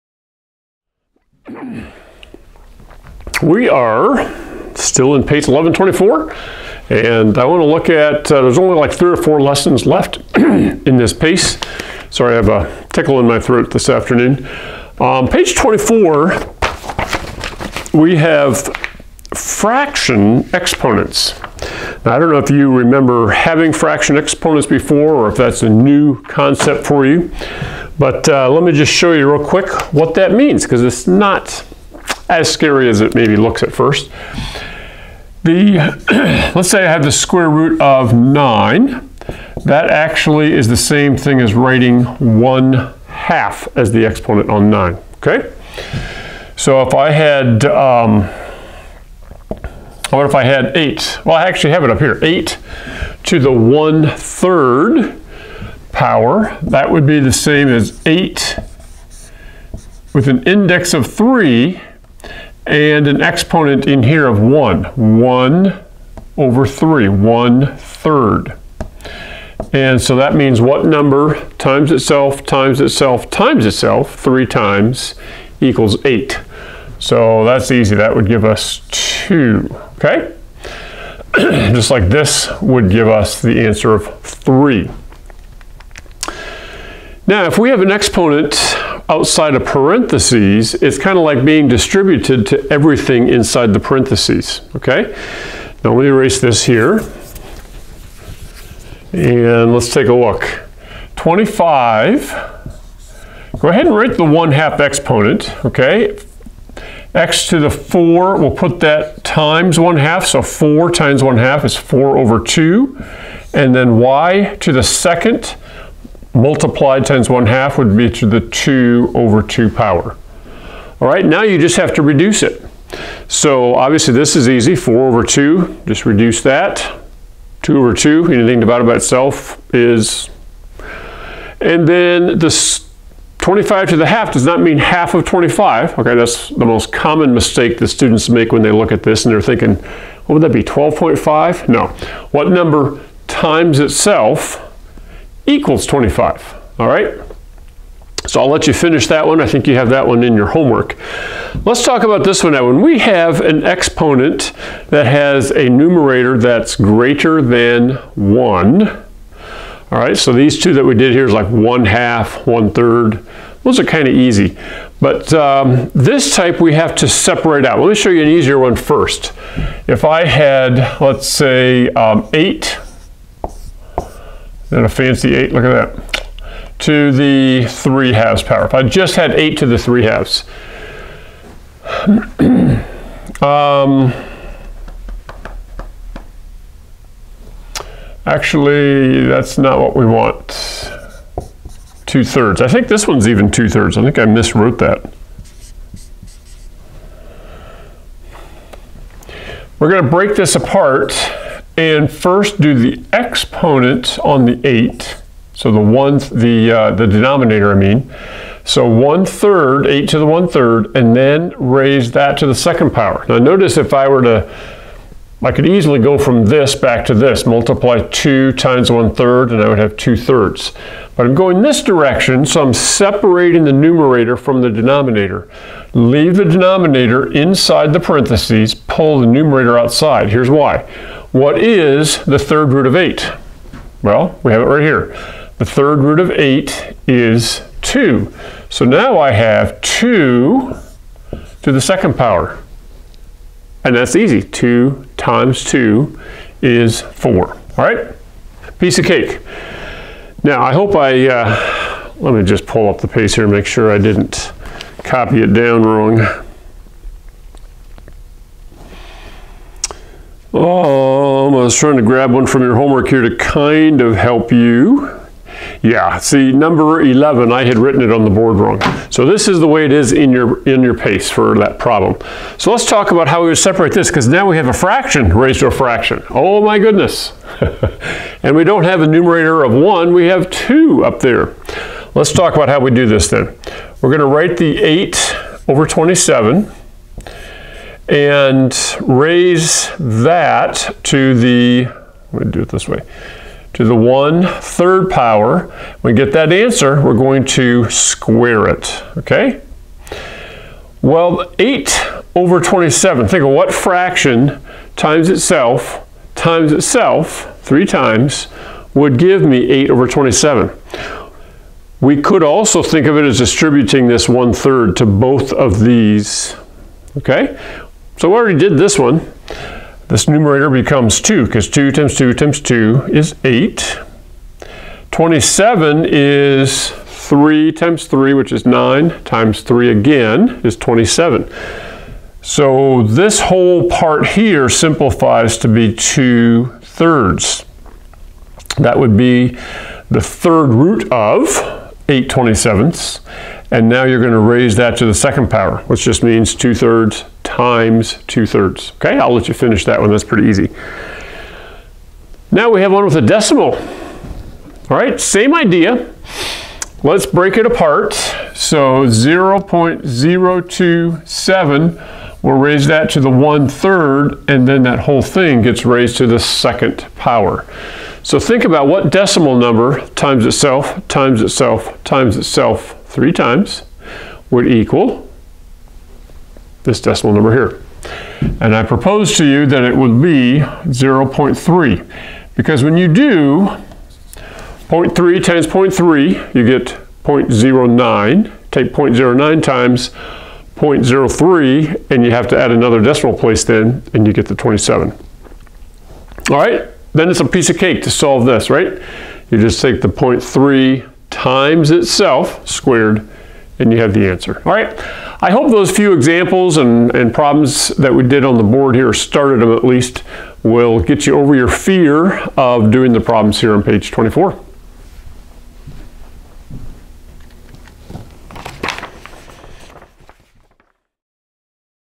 Algebra 2 1124 p 24
This video is a lesson about fractional exponents as covered on pages 23-24 of PACE 1124 of ACE Algebra 2